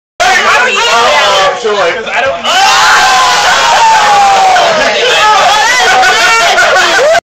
New Year's Eve - The boys had to eat raw eggs to stay over.